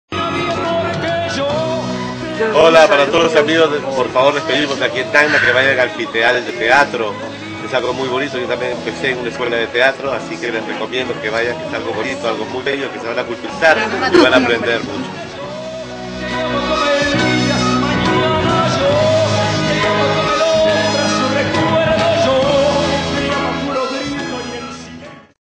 COMERCIAL FITEAL 2012 CON RAFAÉL PERUANO
WALTER POLASTRI, EL "RAFAÉL PERUANO" INVITA A VIVIR EL TEATRO INTENSAMENTE EN EL FITEAL 2012... TE ESPERAMOS